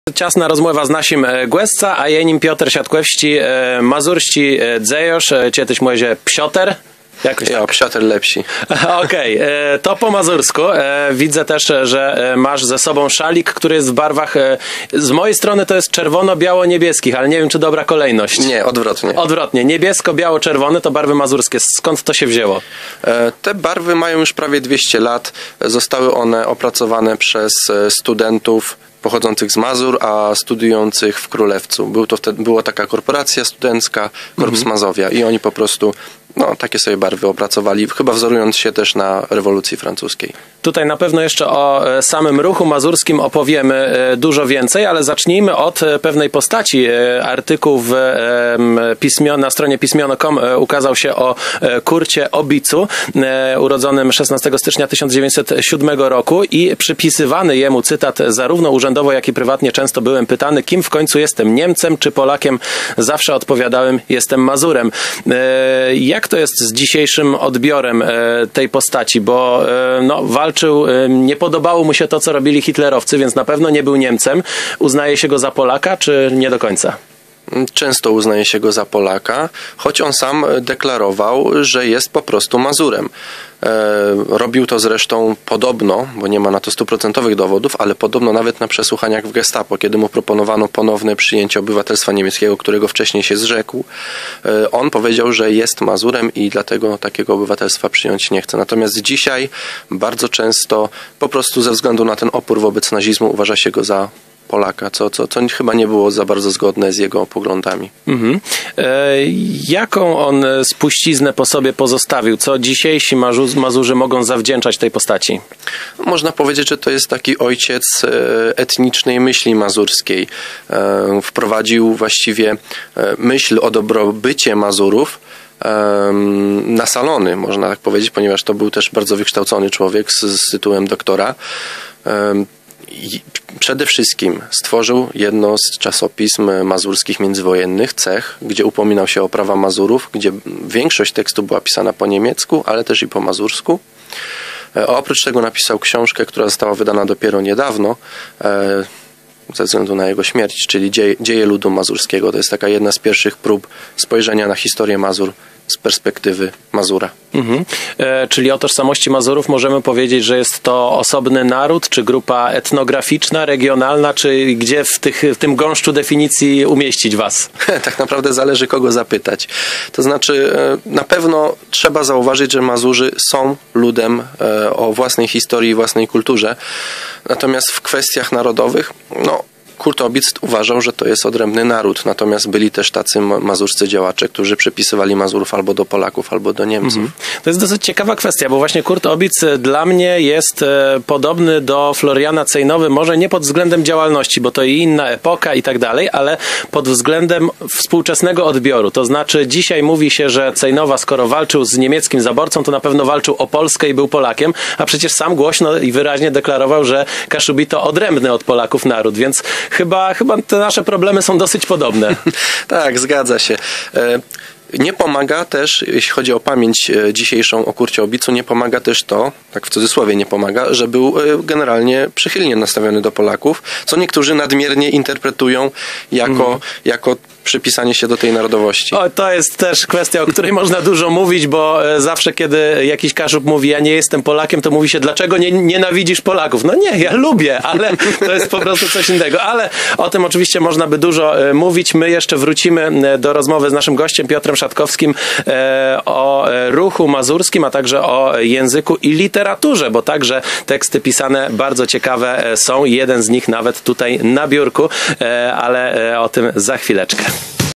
Pioter Szatkòwsczi - Kurt Obitz pitóny ò nôrodnosc gôdôł "Mazur"
Mazurowie mają wiele pòspólnégò z nama, chòcbë w tim, jak sã widzy jich w spòlëznie. Przikładã mòże bëc Kurt Obitz, dzejôrz, jaczégò dzysô sã mô za Pòlôcha, chòc òn pòdsztrichiwôł, że Mazurowie to apartny lud, jistno jak to je z Cenôwą. Czekawą historiã prezeńtëje nama dzysdniowi dzejôrz Pioter Szatkòwsczi.